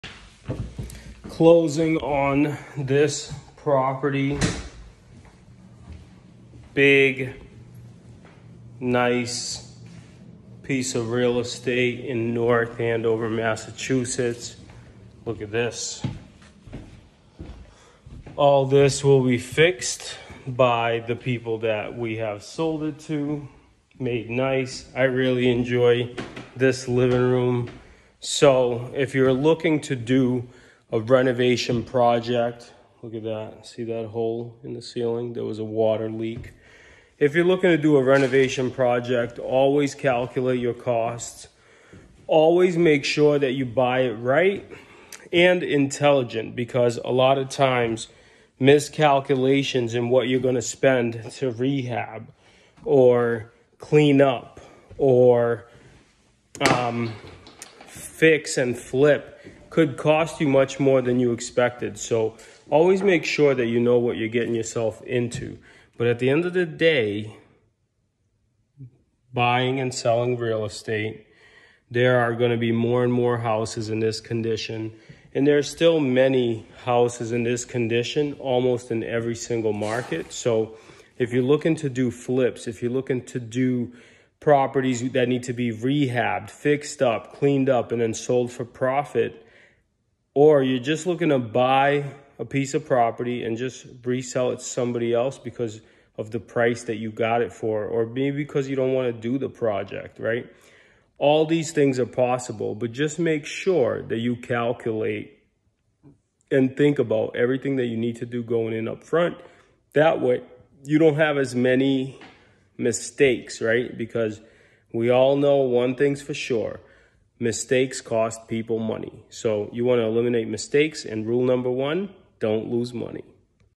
Anne Rd North Andover Sold
Sold another property on Anne Road North Andover 01845.

Overall this property needed a lot of updating great opportunity to do your own finishing’s and furnishings. Great town and the community to be a part of. North Andover middle offers a lot of opportunities to be in nature and also great proximity to the city and to go up to New Hampshire as well. Taking care of a house takes a lot of work and if you are just about done and you wanna sell the home, give me a call and we will be able to help you. Another property sold.